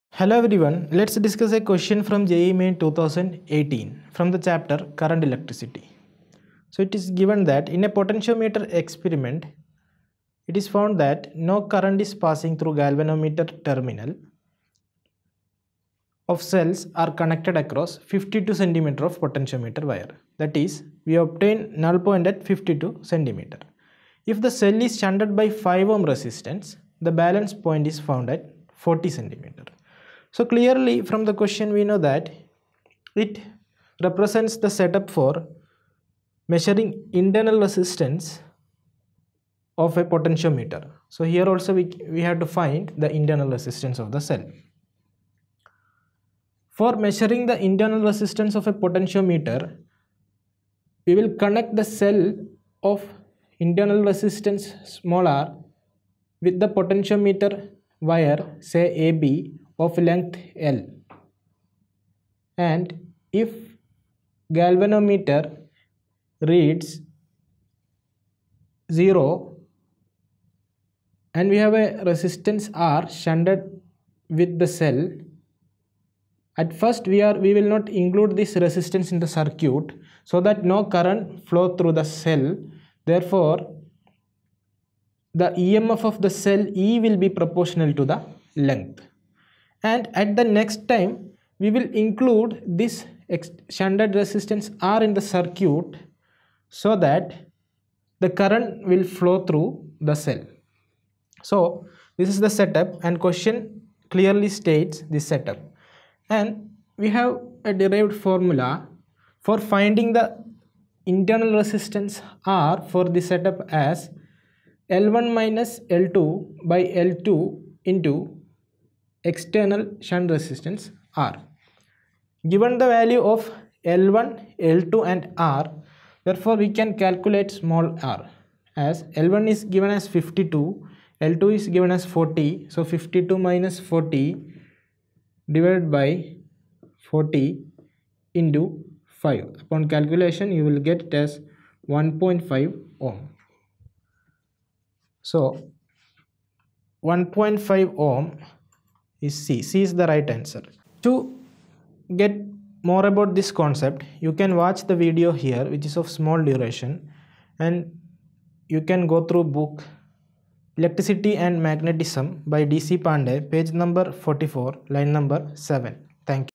JEE Main 2018 Physics Question- In a potentiometer experiment, it is found that no current passes
Target JEE April Detailed Explanation

In a potentiometer experiment, it is found that no current passes through the galvanometer when the terminals of the cell are connected across 52 cm of the potentiometer wire. If the cell is shunted by a resistance of 5 Ω, a balance is found when the cell is connected across 40 cm of the wire. Find the internal resistance of the cell.

    Option 1)   2.5 Ω
    Option 2)   1 Ω
    Option 3)   1.5 Ω
    Option 4)   2 Ω